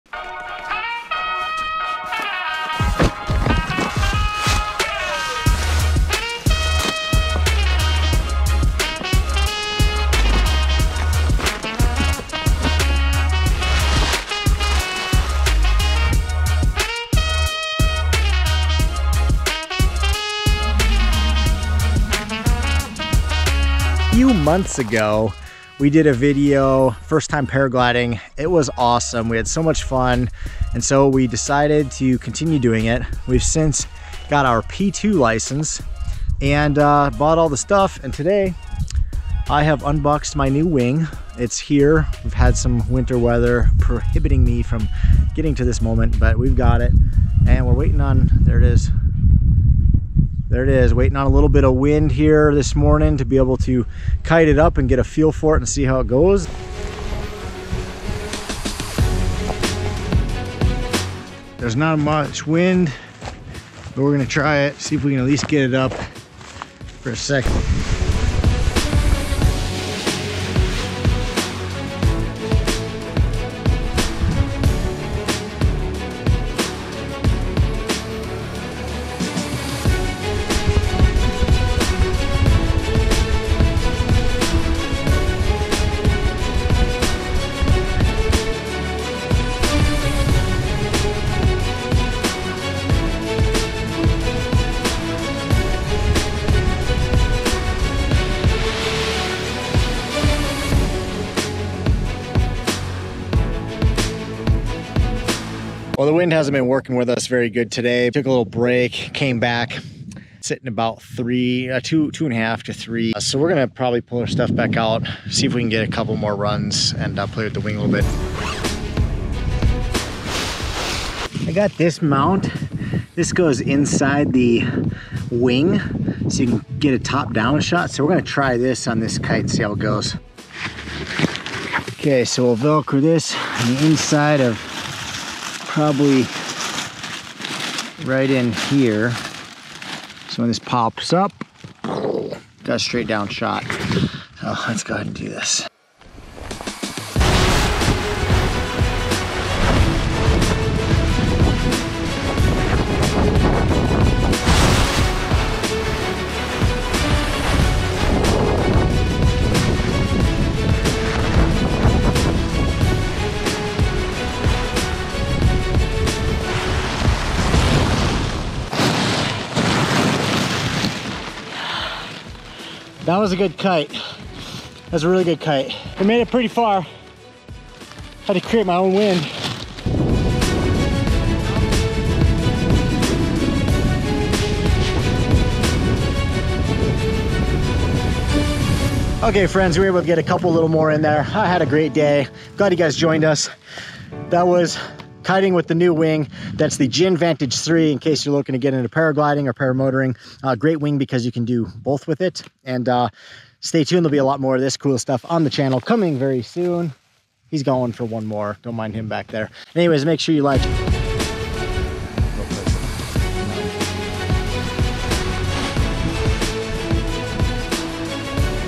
My First Paragliding Wing
Unboxing and kiting my new (and first) Paragliding wing.

PARAMOTOR!!!

GEAR USED!!!
🔸 Wind Meter
🔸 Wind Sock
🔸 GoPro Wing Mount
🔸 GoPro Hero 8
🔸 ND Filters
🔸 SD Cards